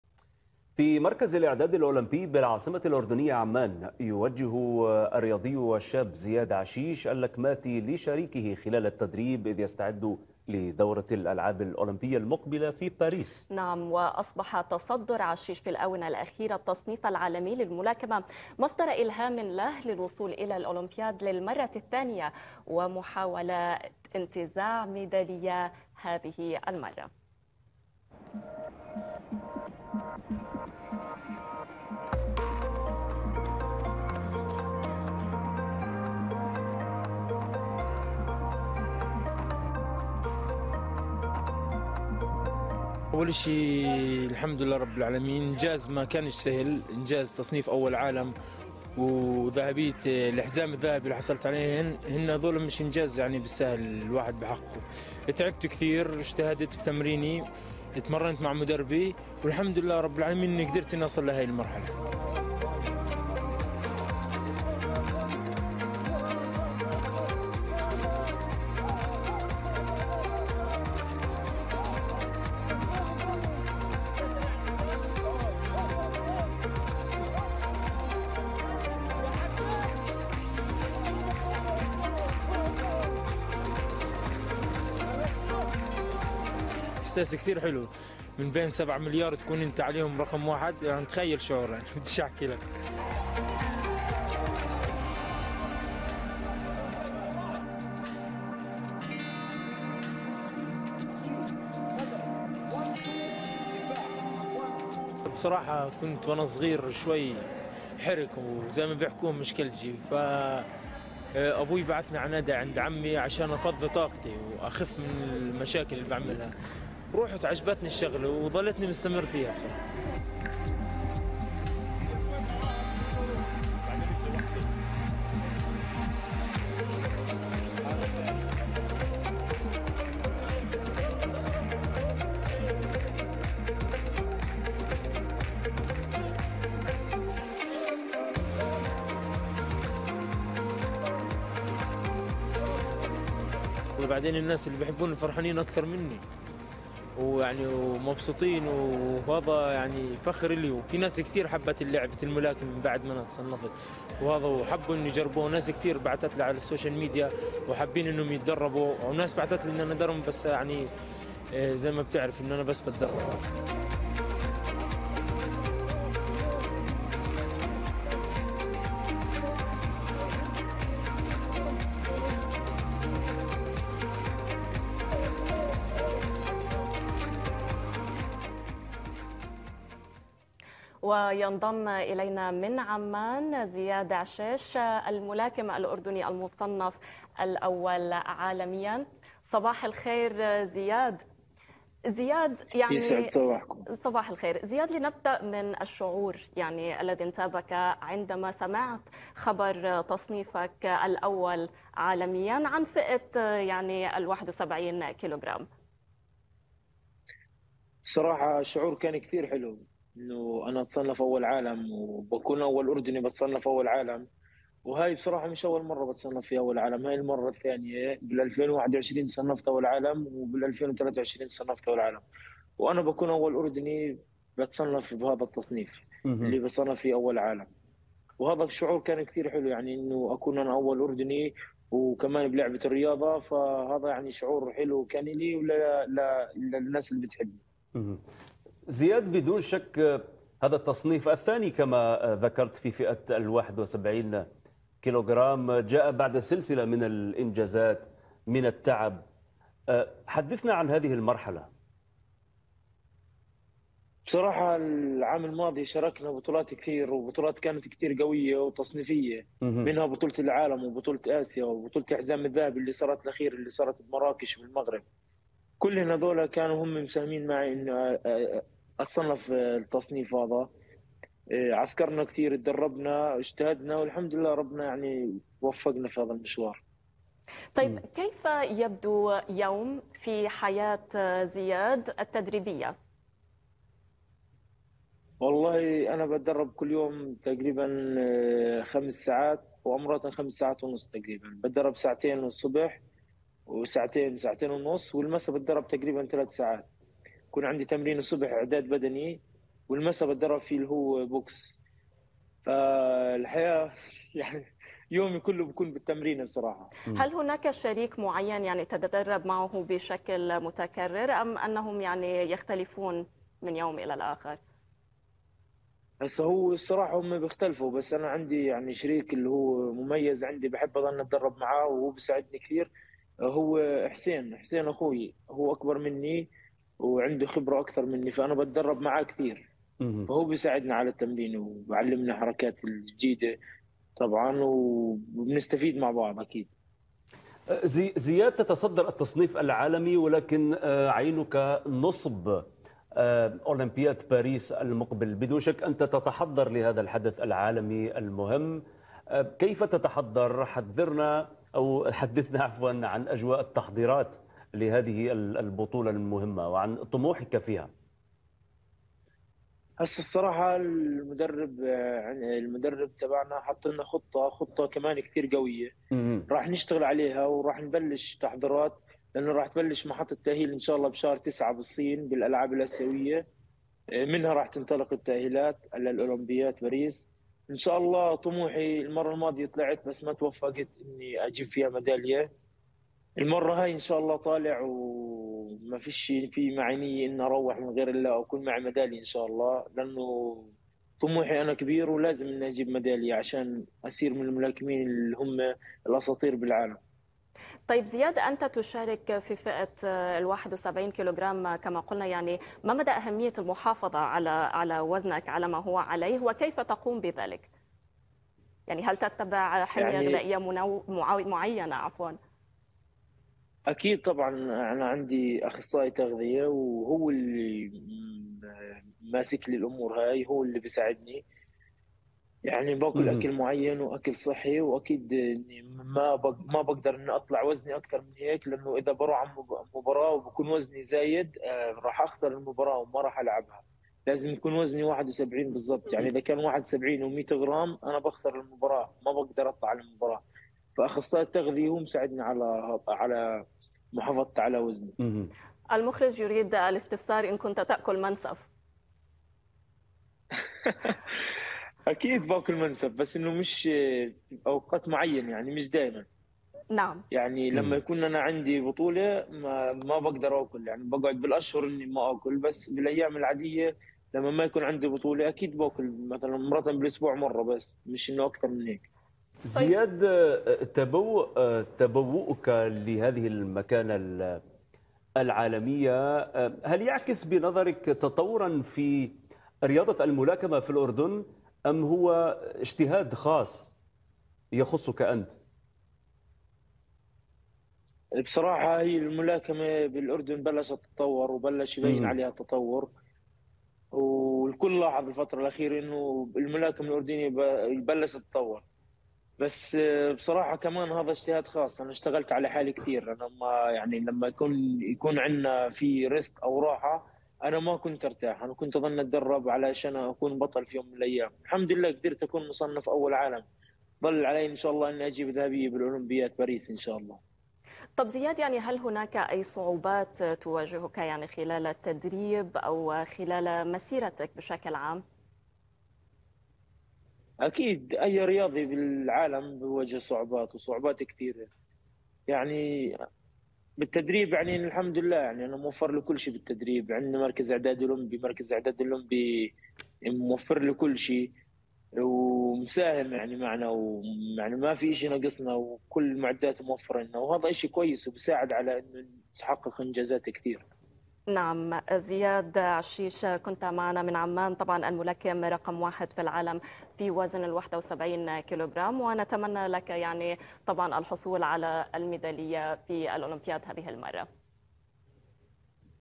الملاكم الأردني زياد عشيش يتصدر التصنيف العالمي .. تعرفوا عليه في هذا اللقاء
تصدر لاعب المنتخب الأردني للملاكمة، زياد عشيش، البالغ من العمر 24 عامًا، قائمة التصنيف الدولي الصادرة عن الاتحاد الدولي للعبة في وزن 71 كغم بعد الإنجازات التي حققها في الفترة الماضية، وكان آخرها حصوله على الميدالية الذهبية في بطولة الحزام الذهبي التي أقيمت في المغرب استعدادا للمشاركة في دورة الألعاب الأولمبية في باريس.